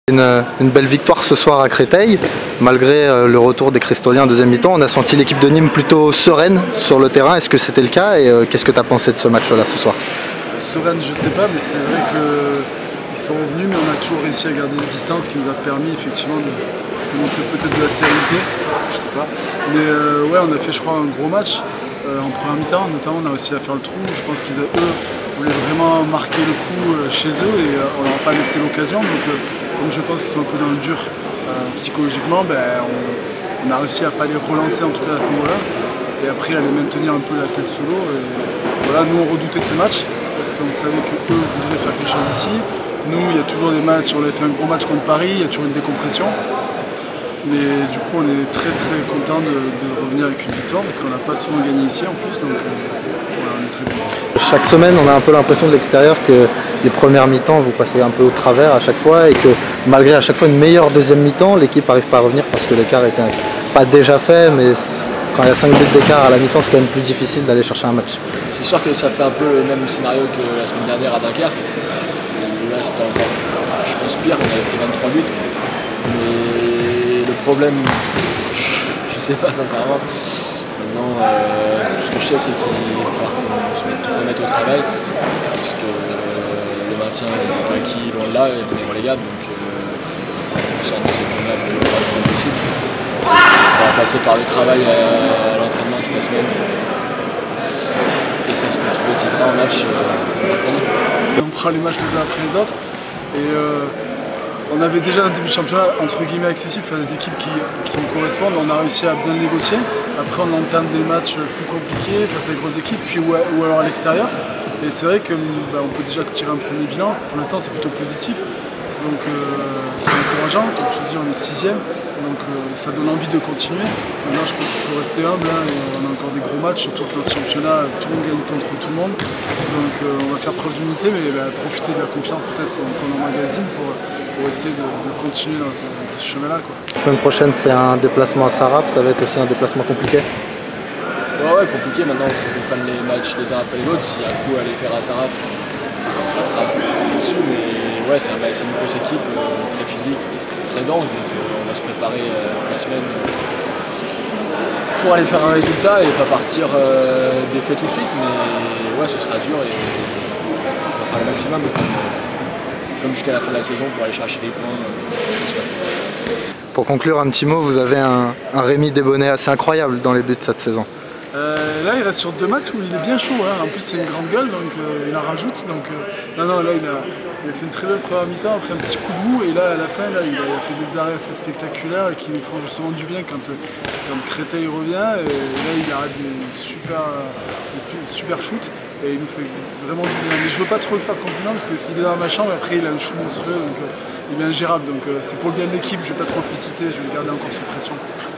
USCHB TV : Réactions après Créteil/Nîmes
Réactions de Jean-Phillippe HAON (Nîmes) et de Lucas FERRANDIER (Créteil) après la rencontre Créteil/Nîmes (25-29)